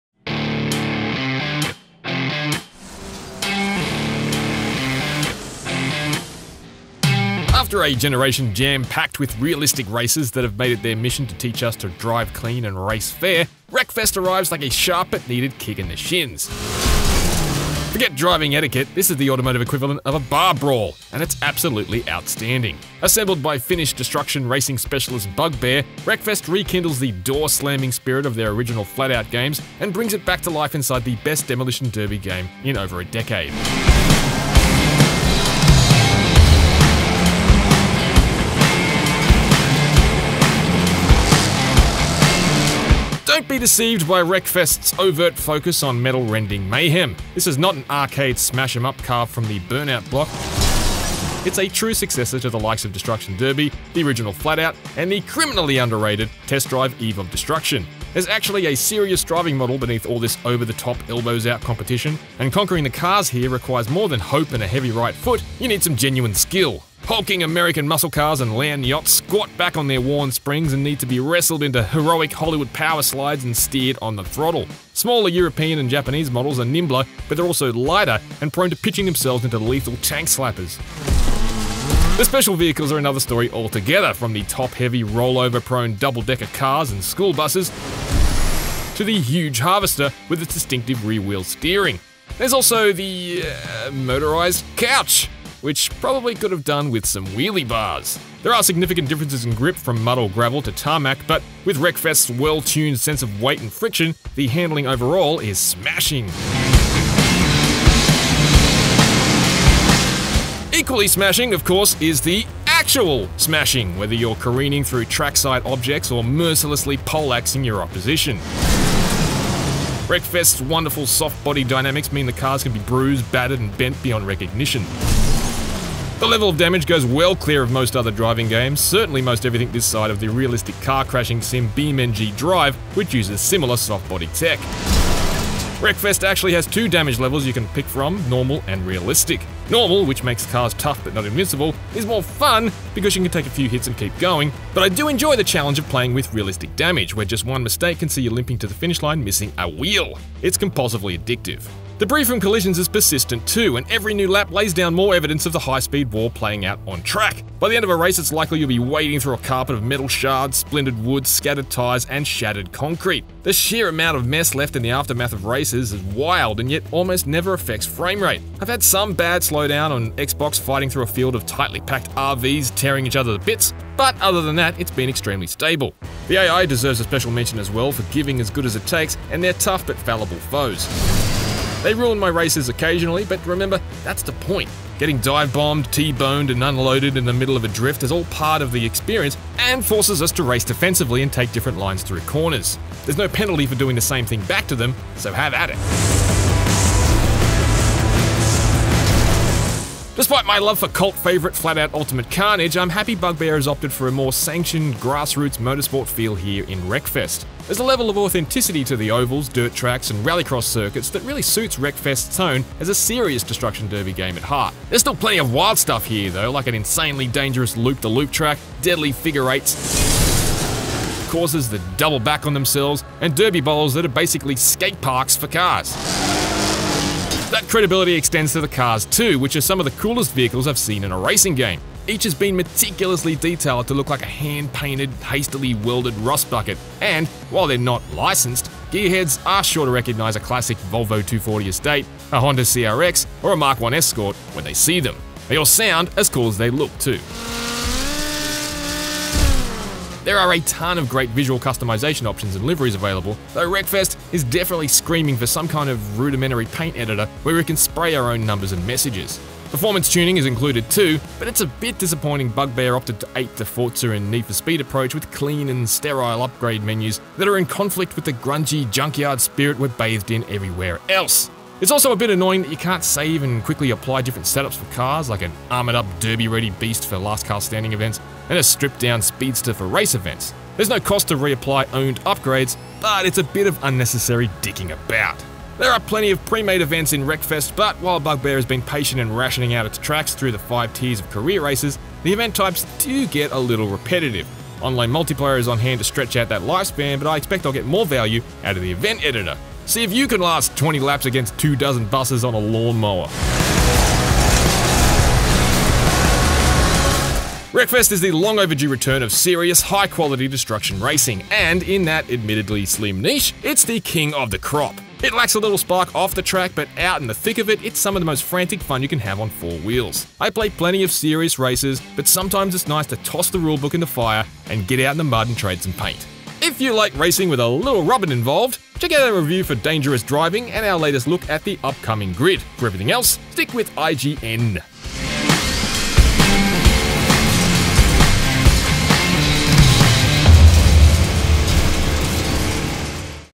Wreckfest Review
Reviewed by Luke Reilly on Xbox One X. Also available on PlayStation 4 and PC.

t